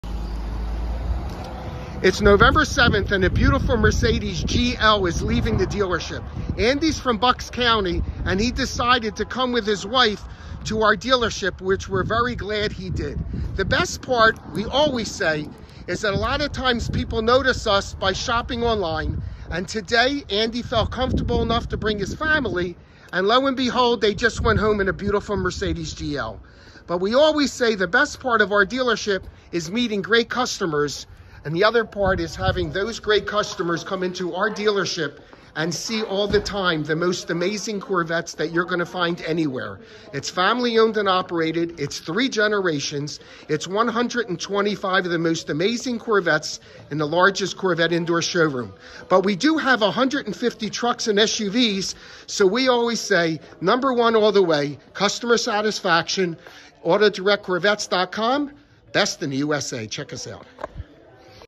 Mercedes Benz SUV
Congratulations Andy on the purchase of this beautiful Mercedes-Benz SUV. It was our pleasure to do business with you. We hope you’re enjoying your new SUV. Best wishes for a great new year!